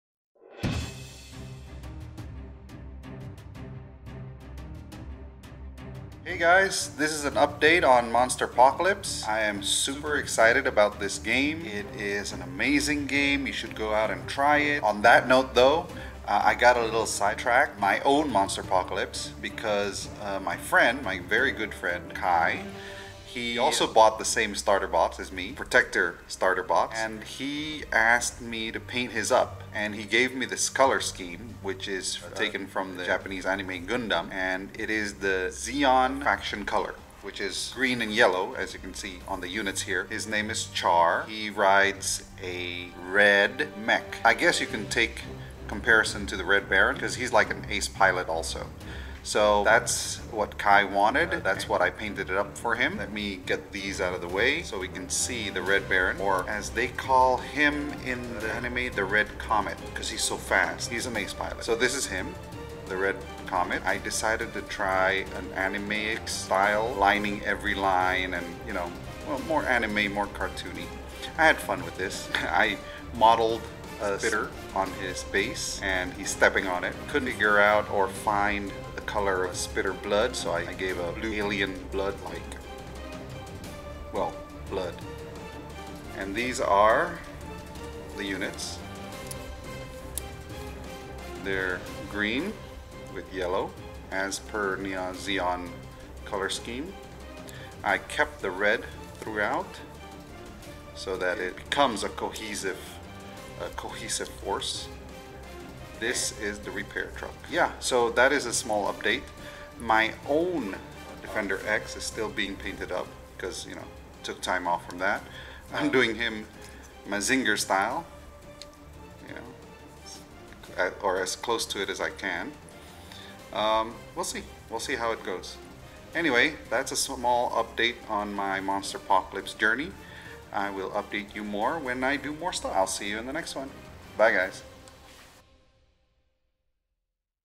Monsterpocalpyse Painting Update
A small update video of my Monsterpocalypse journey, showing off a Defender X that I painted up for my friend

Lemme know what you guys think of him